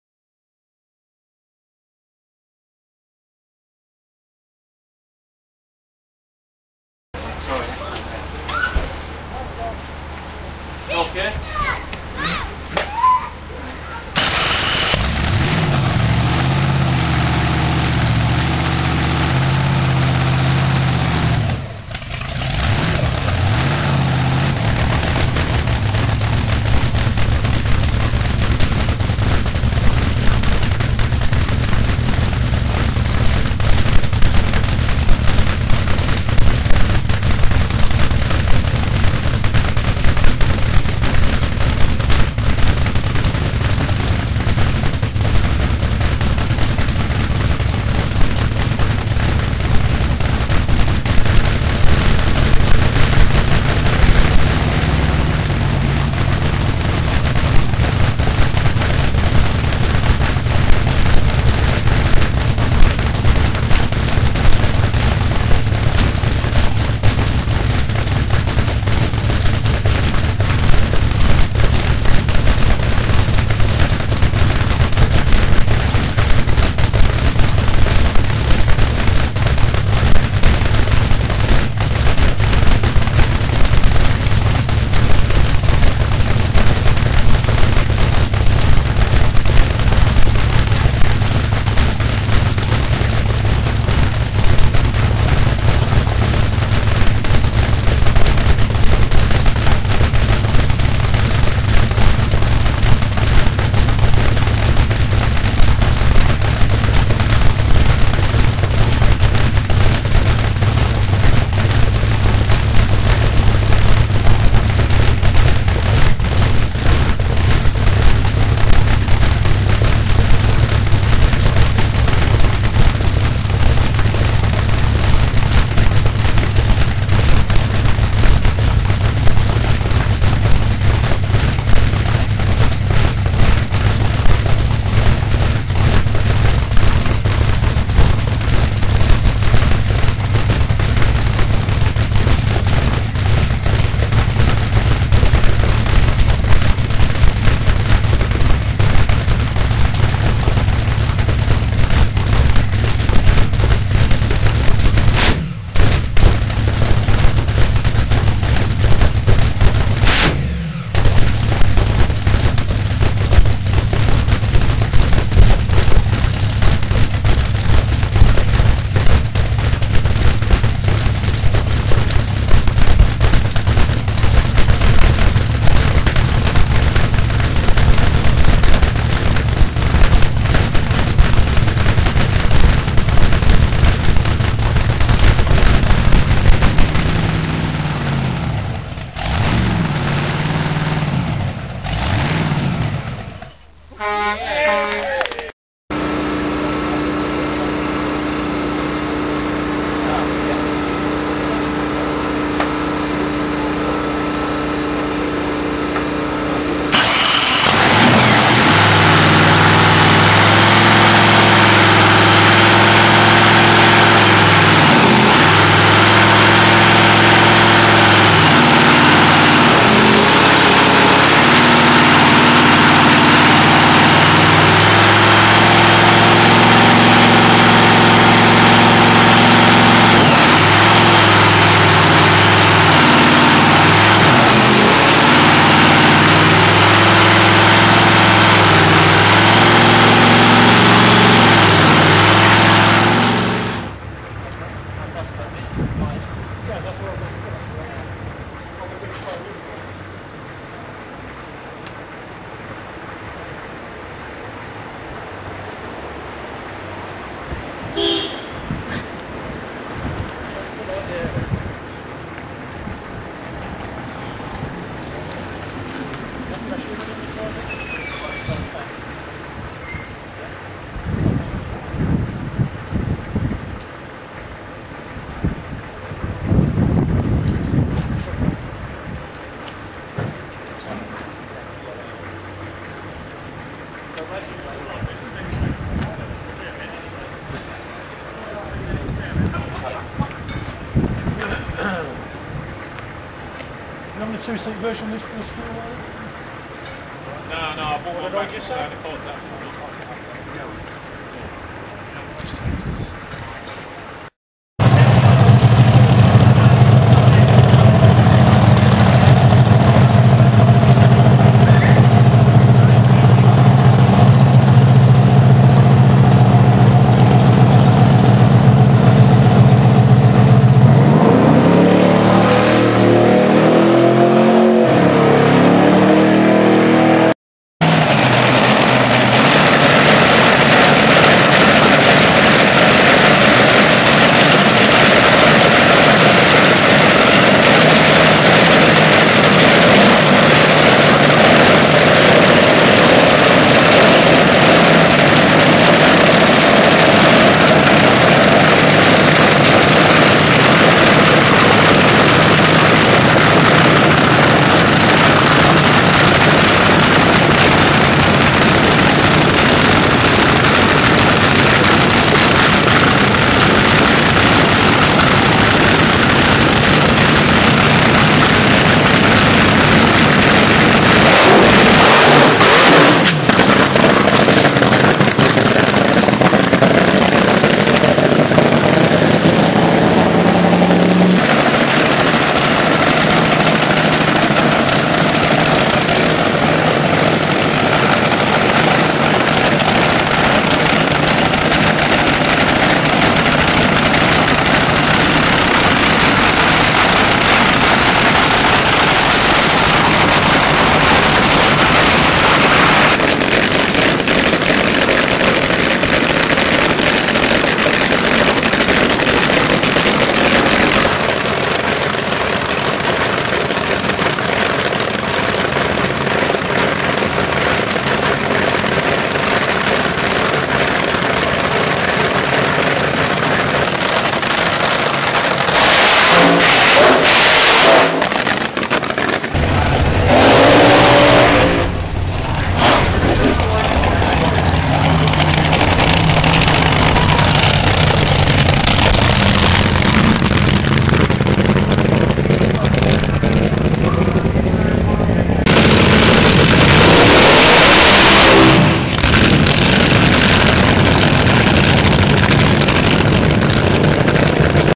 BASSLINE 2008.wmv
Jon Best debut's his blown chevy slingshot on 95% nitro in 2008, AWESOME!!!